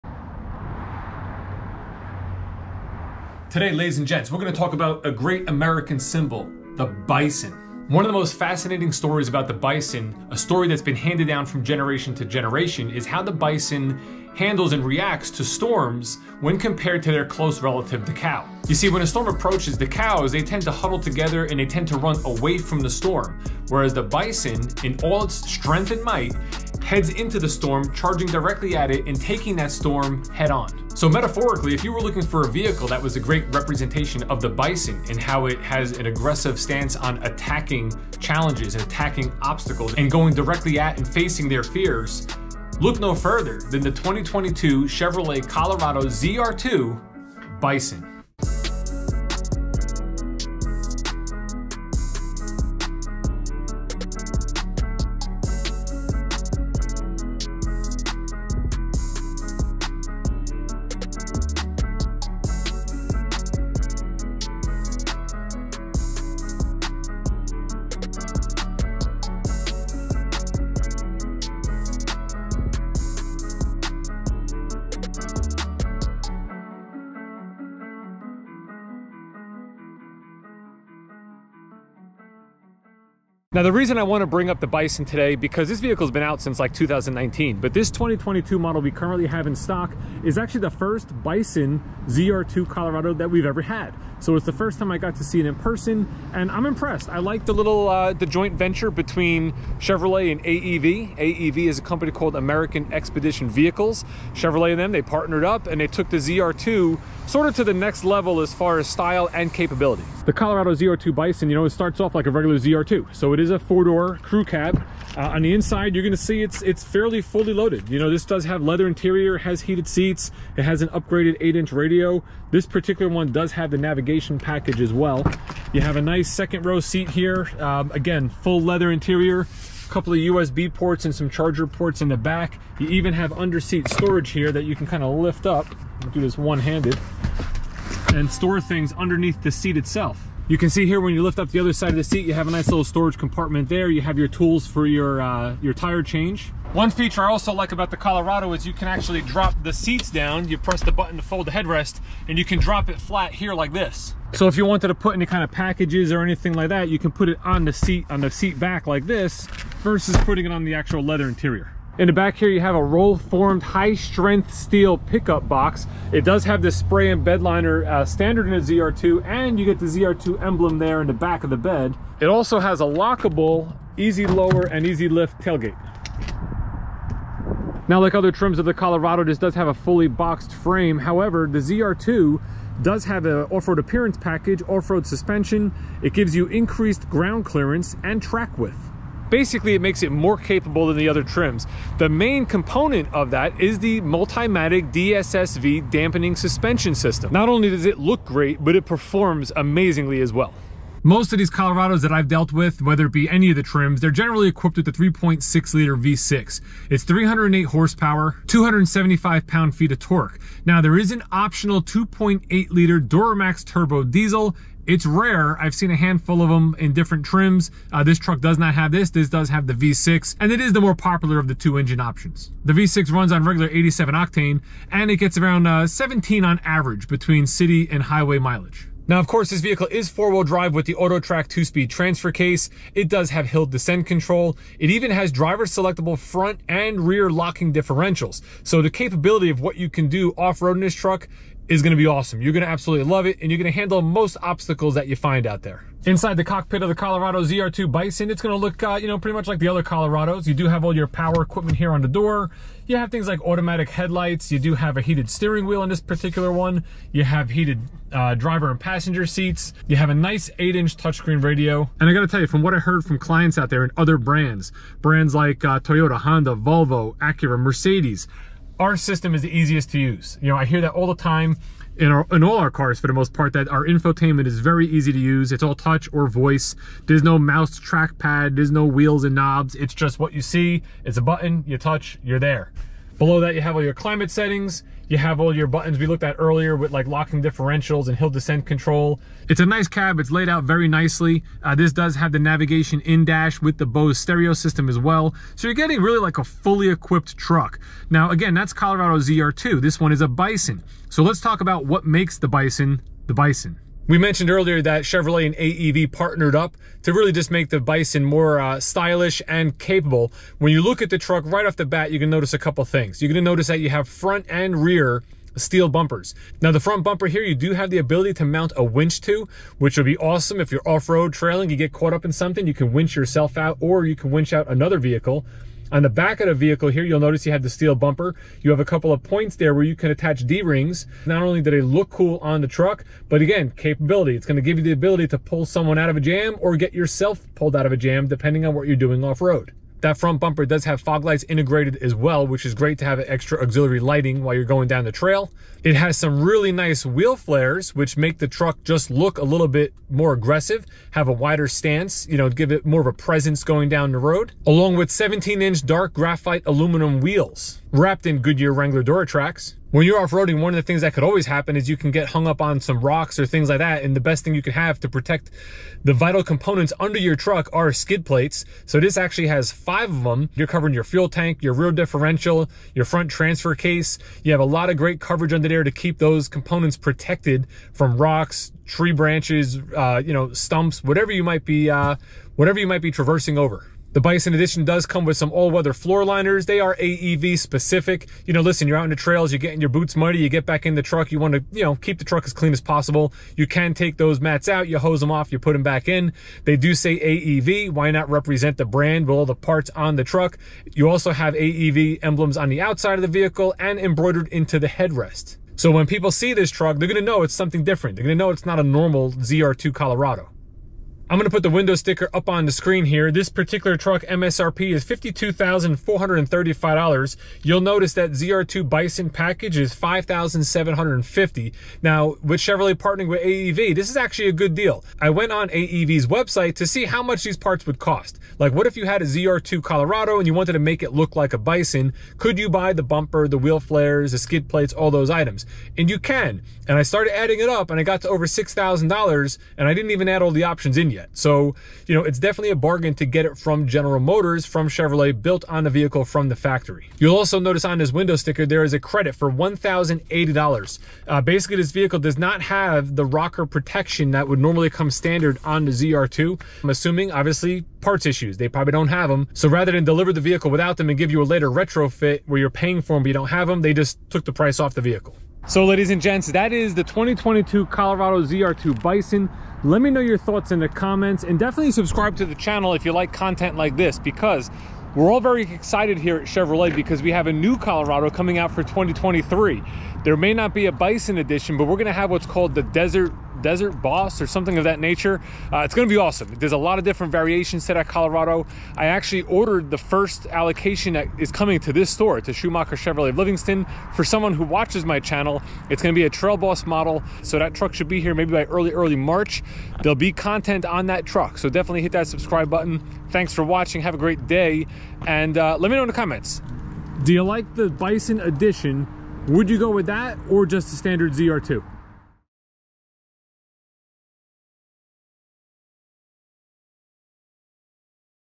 2022 Chevrolet Colorado Zr2 Bison - FOR SALE
This 2022 Chevrolet Colorado Zr2 Bison is the last one we will have before the big changes in the Colorado. If you like this model and live in the NJ area I'd be happy to help out. Just call me, Dave B, at Schumacher Chevrolet of Livingston.

Check out the Dave B logo t-shirt, keychain, and air freshener here on my